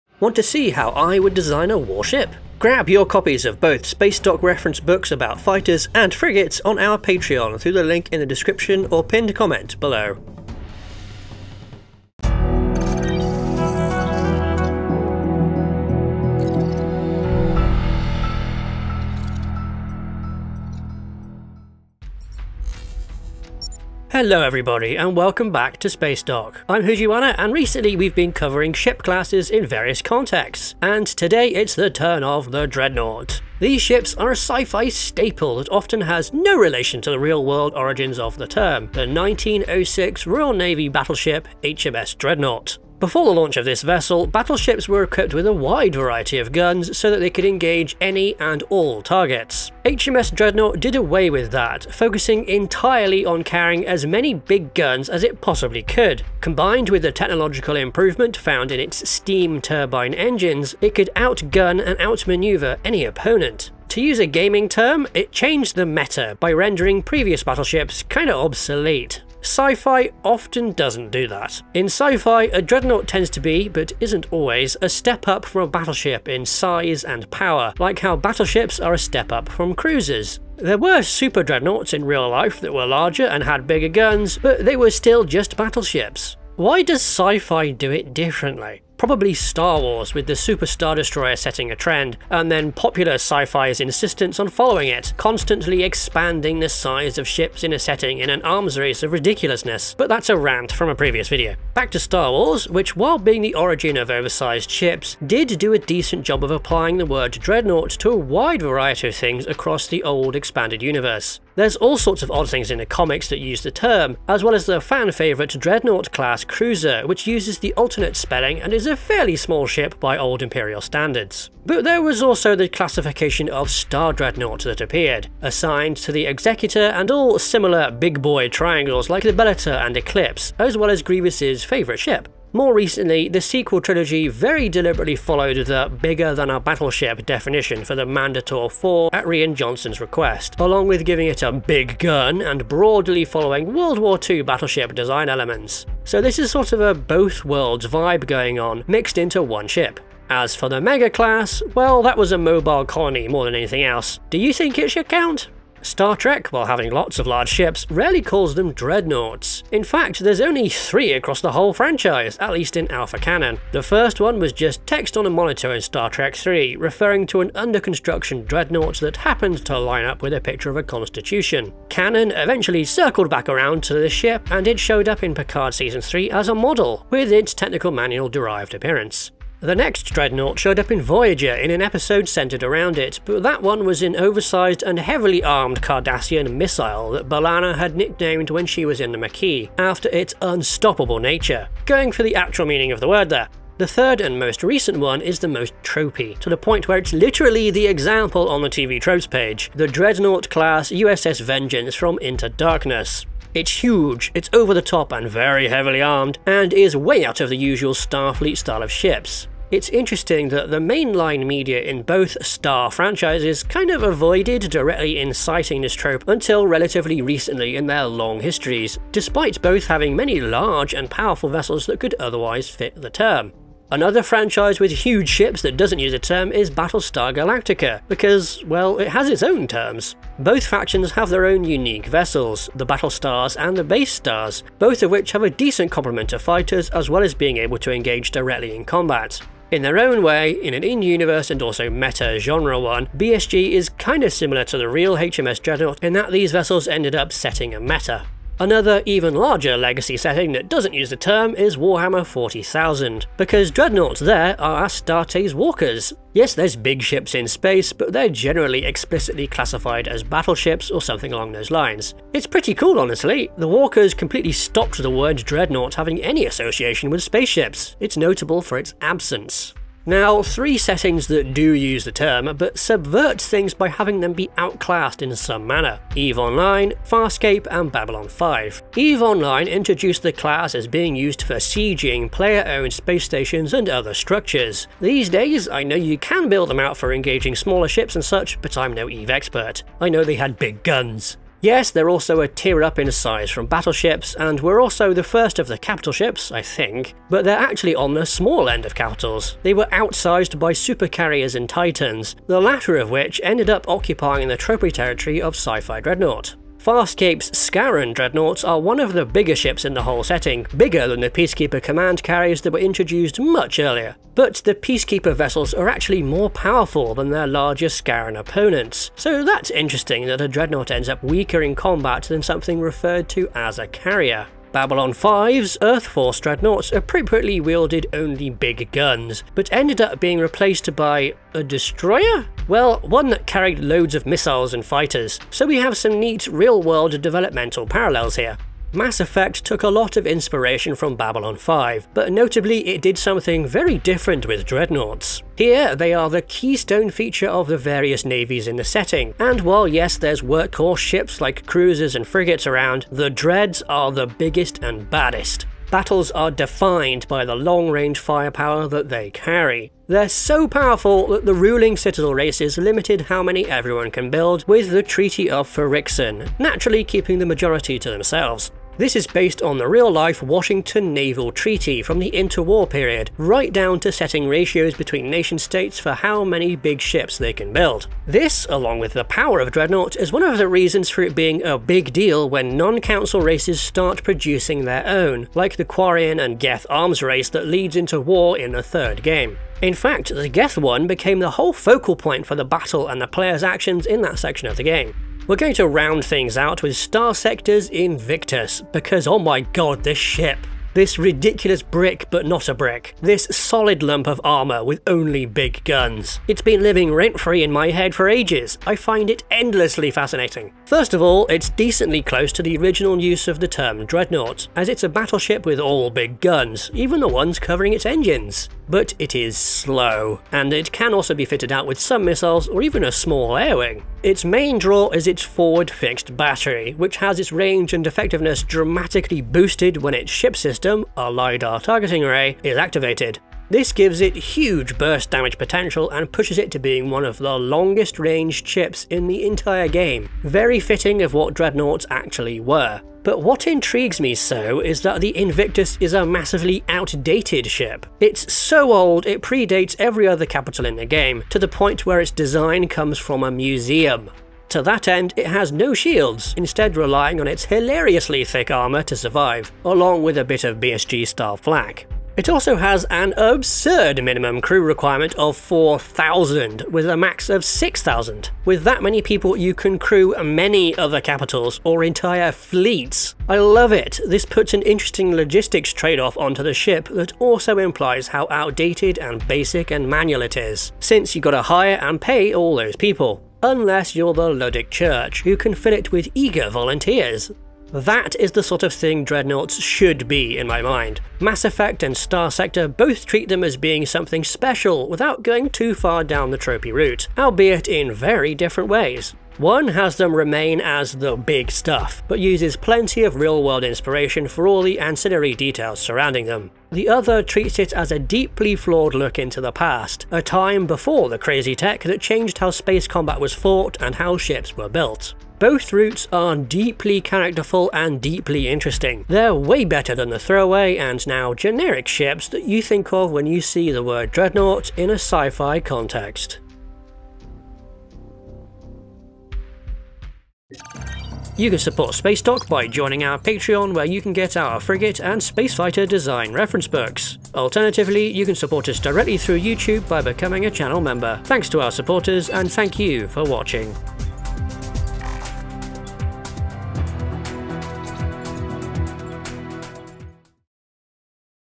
Dreadnoughts in Sci-Fi and History
Get 'Designing a Space Frigate', the latest Official Spacedock Reference Book,

Spacedock delves into the mighty dreadnoughts of fact and fiction.